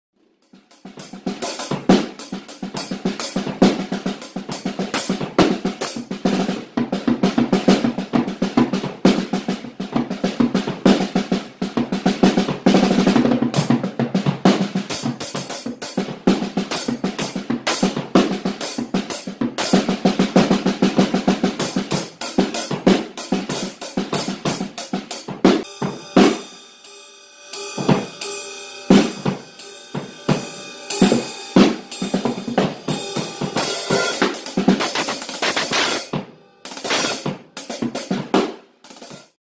Zen drums
I love Drumming with a passion ! this is just a little mess around practice with a funny bit at the end. I will be posting more to my page regarding my instrumentation whether it be practice or upcoming recordings